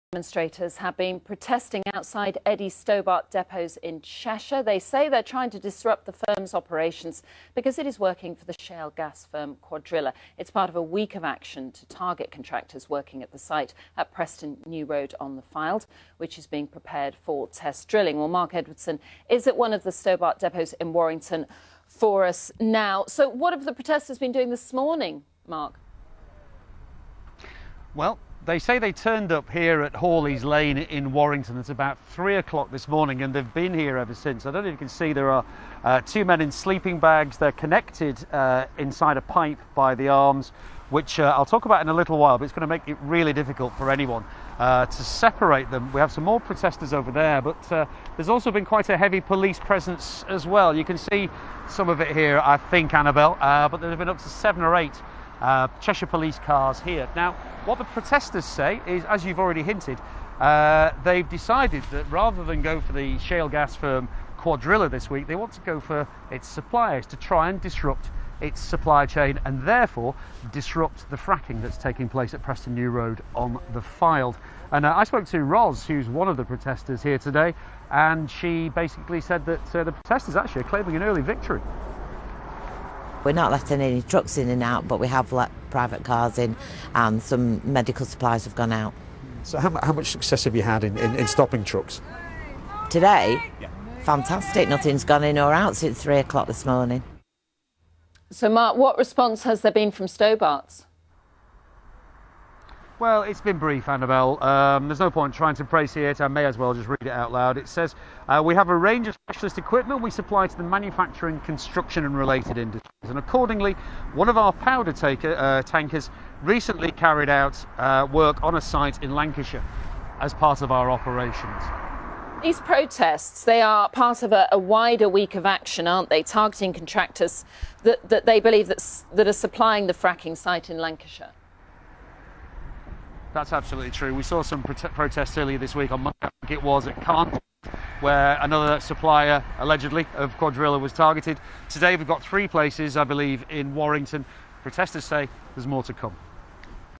FRACK OFF EDDIE STOBART
Eddie Stobart trucks back to the stock market with £550m Aim float.
The company, known for its green and red lorries, has announced plans to list on the London Stock Exchange’s junior market next month, seeking to raise £130m.